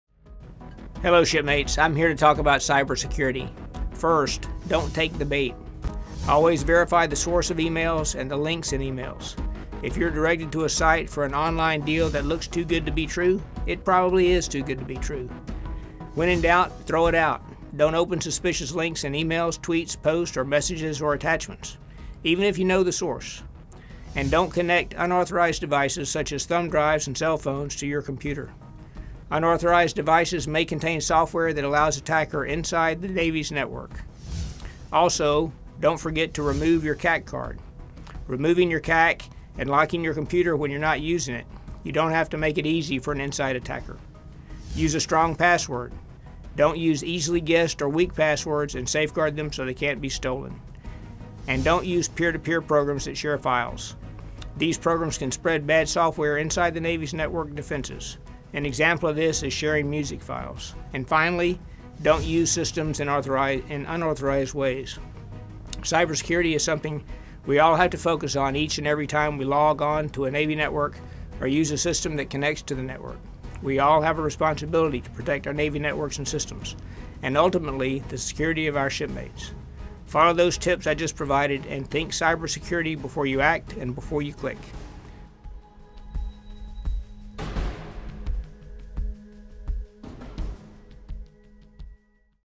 Protecting Yourself Against Cyber Attack
Adm. Ted Branch speaks about the cyber threats facing the US Navy today, and what you can do to help fight back.